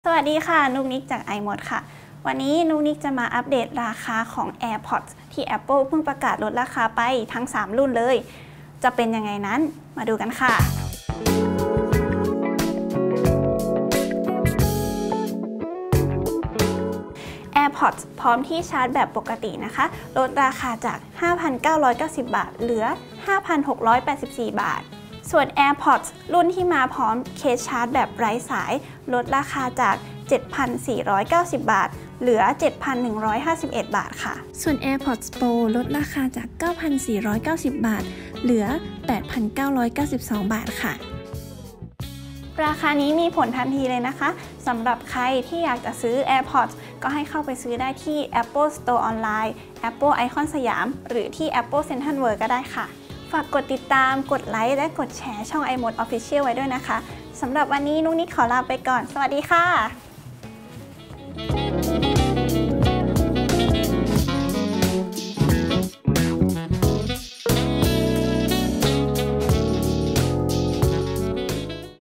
AirPods ลดราคาทุกรุ่น มีผลทันที - iMoD
Apple ลดราคา AirPods ทุกรุ่น ดังนี้

✅  AirPods พร้อมเคสชาร์จ เหลือ 5,684 บาท (จากเดิม 5,990 บาท)
✅  AirPods พร้อมเคสชาร์จไร้สาย เหลือ 7,151 บาท (จากเดิม 7,490 บาท)
✅  AirPods Pro เหลือ 8,992 บาท (จากเดิม 9,490 บาท)

🛒  หน้าร้าน Apple Store ทั้งสองสาขา : Apple Iconsiam, Apple Central World

บทความที่เกี่ยวข้อง

ℹ️ AirPods ทุกรุ่นลดราคา มีผลทันที